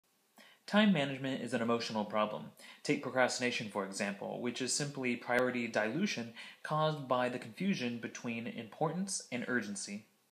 Time Management is an Emotional Problem
Timemanagement is an emotional problem. Take procrastination for example, which is simply priority dilution  caused by the confusion between importance and urgency.